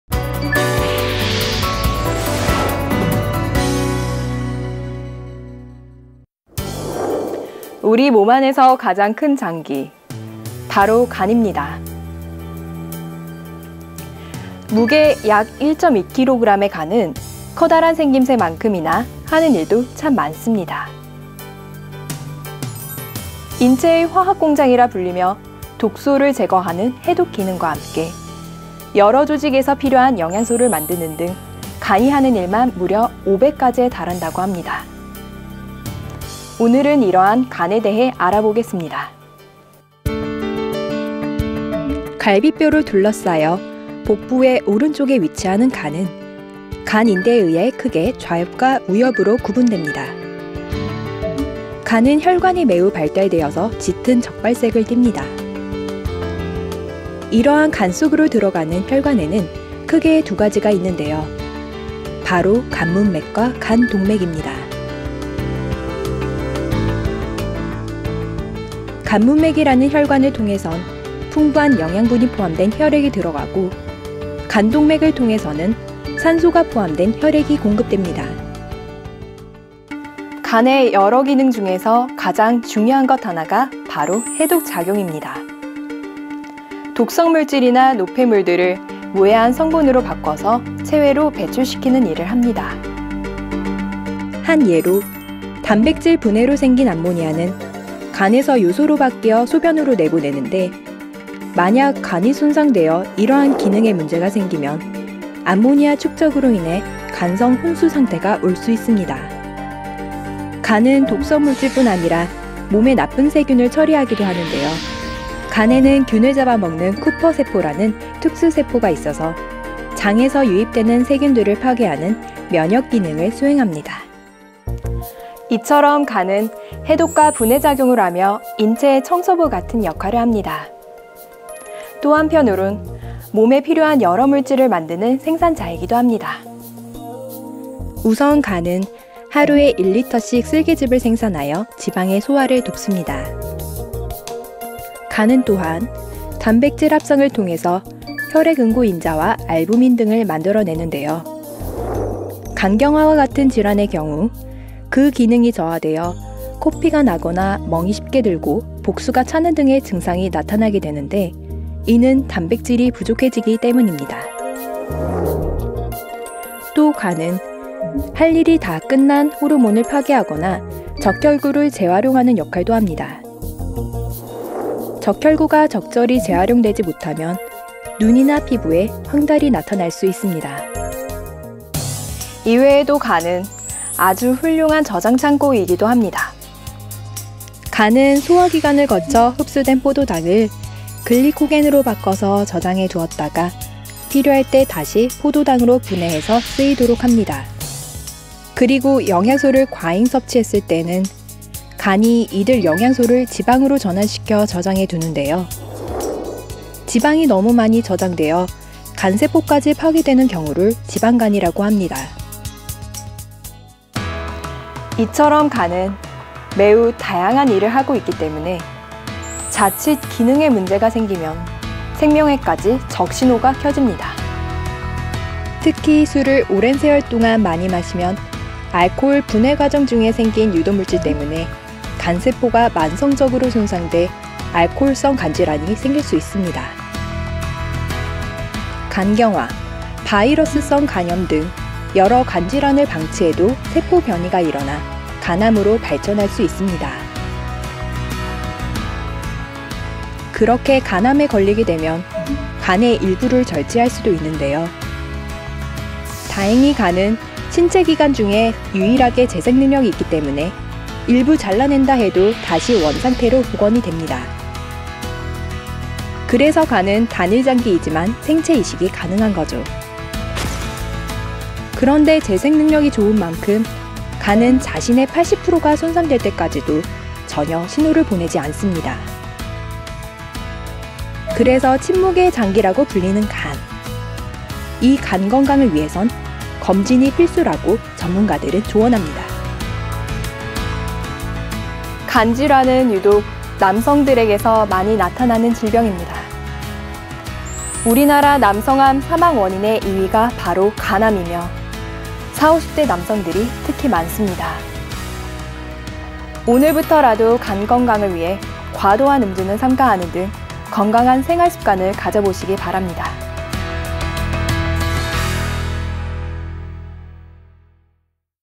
간 [내 몸 둘러보기]
우리 몸 안에서 가장 큰 장기
바로 간입니다..

무게 약 1.2kg의 간은
커다란 생김새만큼이나 하는 일도 참 많습니다.

‘인체의 화학공장’이라 불리며
독소를 제거하는 해독기능과 함께
여러 조직에서 필요한 영양소를 만드는 등
간이 하는 일만 무려 500가지에 달한다고 합니다..

오늘은 이러한 ‘간’에 대해 알아 보겠습니다.

갈비뼈로 둘러싸여 복부의 오른쪽에 위치하는 간은
간(겸상)인대에 의해 크게 좌엽과 우엽 2개로
구분됩니다.

간은 혈관이 매우 발달되어서
짙은 적갈색을 띕니다.

이러한 간 속으로 들어가는 혈관에는 크게 두 가지가 있는데요.
바로 간문맥과 간동맥입니다.

간문맥이라는 혈관을 통해선
풍부한 영양분이 포함된 혈액이 들어가고,
간동맥을 통해서는 산소가 포함된 혈액이 공급됩니다.

간의 여러 기능 중에서
가장 중요한 것 하나가 바로 해독작용입니다.

독성물질이나 노폐물들을 무해한 성분으로 바꿔서
체외로 배출시키는 일을 합니다.

한 예로, 단백질 분해로 생긴 암모니아는
간에서 요소로 바뀌어 소변으로 내보내는데,
만약 간이 손상되어 이러한 기능에 문제가 생기면
암모니아 축적으로 인해 간성 혼수 상태가 올 수 있습니다.

간은 독성물질뿐 아니라, 몸에 나쁜 세균을 처리하기도 하는데요.

간에는 균을 잡아먹는 ‘쿠퍼세포’라는 특수 세포가 있어서
장에서 유입되는 세균들을 파괴하는 면역기능을 수행합니다.

이처럼 간은 ‘해독’과 ‘분해’ 작용을 하며 인체의 청소부 같은
역할을 합니다.
또, 한편으론 몸에 필요한 여러 물질을  만드는
생산자이기도 합니다.

우선 간은 하루에 1리터씩 쓸개즙을 생산하여
지방의 소화를 돕습니다.

간은 또한, 단백질 합성을 통해서
혈액응고인자와 알부민 등을 만들어 내는데요.
간경화와 같은 질환의 경우 그 기능이 저하되어 코피가 나거나 멍이 쉽게 들고 복수가 차는 등의 증상이 나타나게 되는데 이는 단백질이 부족해 지기 때문입니다.

또, 간은
할 일이 다 끝난 호르몬을 파괴하거나 적혈구를 재활용하는 역할도 합니다.
적혈구가 적절히 재활용되지 못하면
눈이나 피부에 황달이 나타날 수 있습니다.

이외에도, ‘간’은
아주 훌륭한 저장창고이기도 합니다.

간은 소화기관을 거쳐 흡수된 포도당을
글리코겐으로 바꿔서 저장해두었다가
필요할 때 다시 포도당으로 분해해 쓰이도록 합니다.

그리고 영양소를 과잉 섭취했을 때는
간이 이들 영양소를 지방으로 전환시켜 저장해 두는데요.
지방이 너무 많이 저장되어 간세포까지 파괴되는 경우를 지방간이라고 합니다.

이처럼 간은 매우 다양한 일을 하고 있기 때문에
자칫 기능에 문제가 생기면 생명에까지 적신호가 켜집니다..

특히 술을 오랜 세월 동안 너무 많이 마시면
알코올 분해과정 중에 생긴 유독물질 때문에
간세포가 만성적으로 손상돼
알코올성 간질환이 생길 수가 있고요.

간경화, 바이러스성 간염 등 여러 간질환을 방치해도
세포변이가 일어나서 간암으로 발전할 수 있습니다.

그렇게 간암에 걸리게 되면 간의 일부를 절제할 수도 있는데요.
다행히 간은 신체기관 중에서 유일하게 재생능력이 있기 때문에
일부 잘라낸다 해도 다시 원상태로 복원이 됩니다.
그래서 간은 단일장기지만, 생체이식이 가능한 거죠.

그런데 재생능력이 좋은 만큼, 간은
자신의 80%가 손상될 때까지도 전혀 신호를 보내지 않습니다.
환자에겐 아무런 자각증세가 없는데요.

그래서 ‘침묵의 장기’라고 불리는 간.
이 간 건강을 위해선 검진이 필수라고 전문가들은 조언합니다.

간 질환은 유독 남성들에게서 많이 나타나는 질병입니다.
우리나라 남성 암 사망 원인의 2위가 바로 간암이며,
40-50대 남성들이 특히 많습니다.

오늘부터라도 간 건강을 위해
과도한 음주는 삼가는 등
건강한 생활습관을 가져보시기 바랍니다.